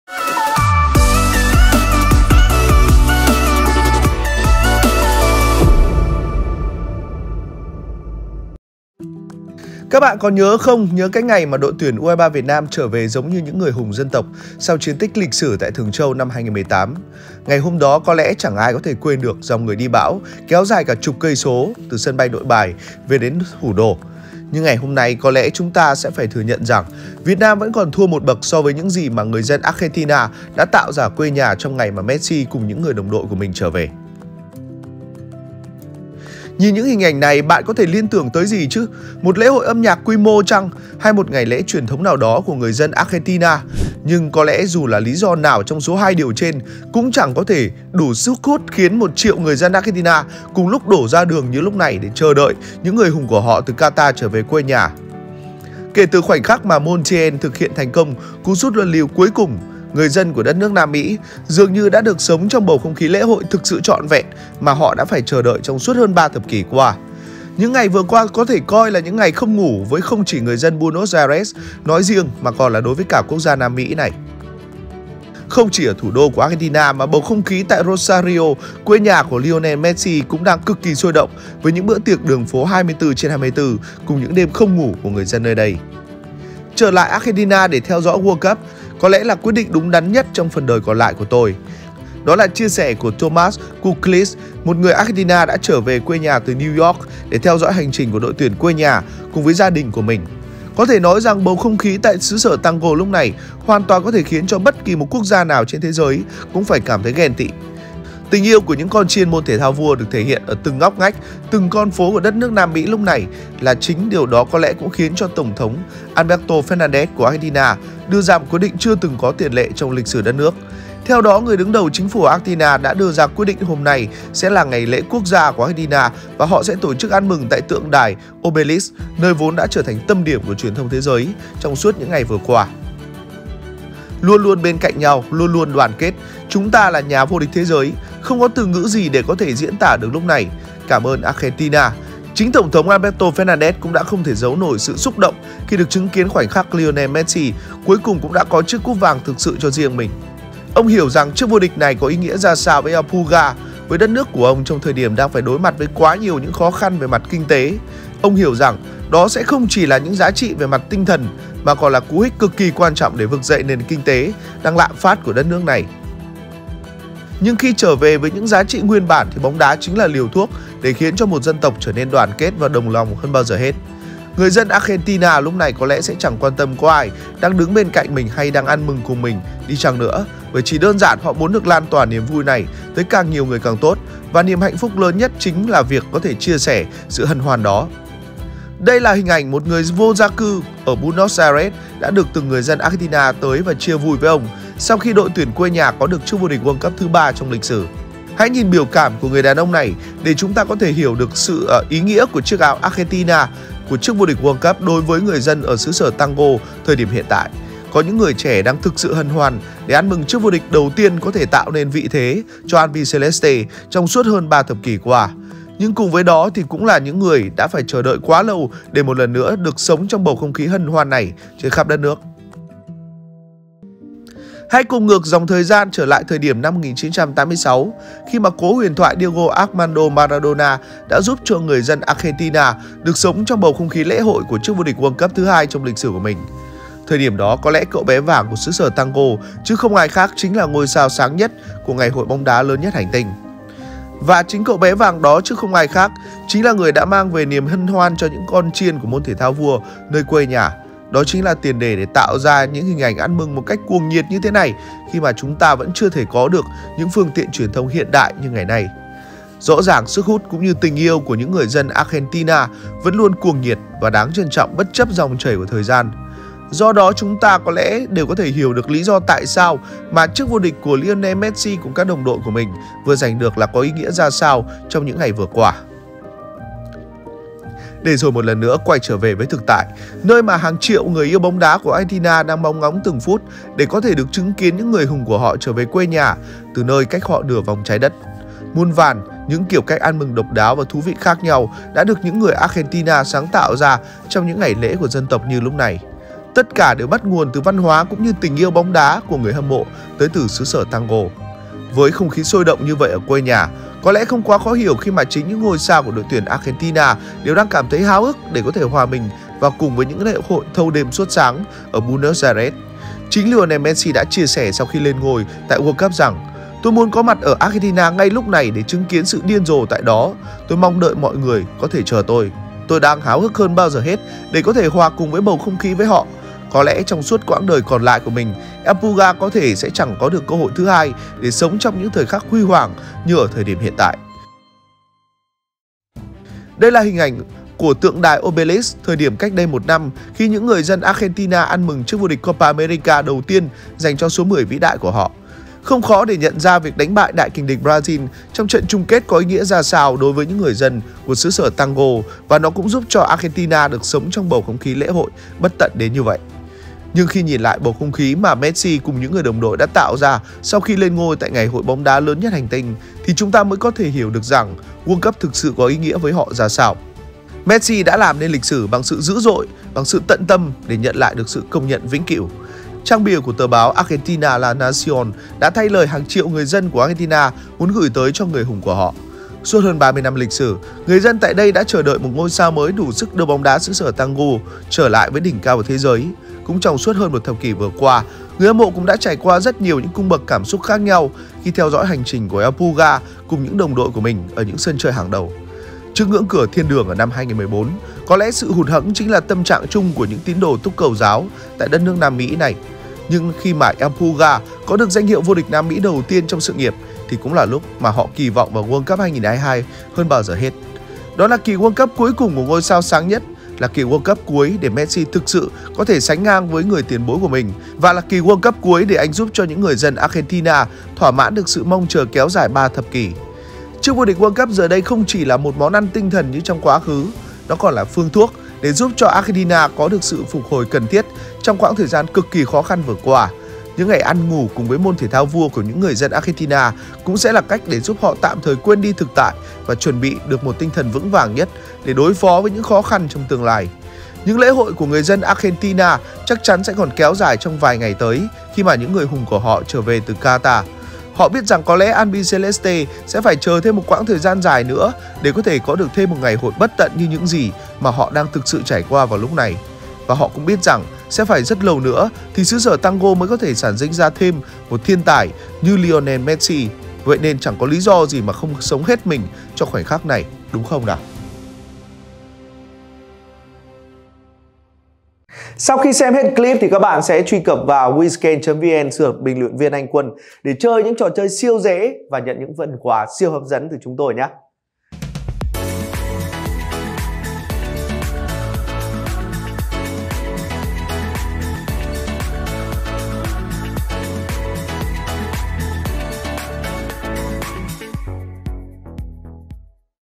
SỰ CUỒNG NHIỆT CỦA BUENOS AIRES TRONG NGÀY LỊCH SỬ ĐÓN MESSI
Dự đoán kết quả các trận đấu để nhận quà tặng từ BLV Anh Quân và Bí Ẩn Ozon tại

Những lễ hội của người dân Argentina chắc chắn sẽ còn kéo dài trong vài ngày tới khi mà những người hùng của họ trở về từ Qatar. Họ biết rằng có lẽ Albiceleste sẽ phải chờ thêm một quãng thời gian dài nữa để có thể có được thêm một ngày hội bất tận như những gì mà họ đang thực sự trải qua lúc này. Và họ cũng biết rằng sẽ phải rất lâu nữa thì xứ sở tango mới có thể sản sinh ra thêm 1 thiên tài như Lionel Messi. Vậy nên chẳng có lý do gì mà không sống hết mình cho khoảnh khắc này đúng không nào.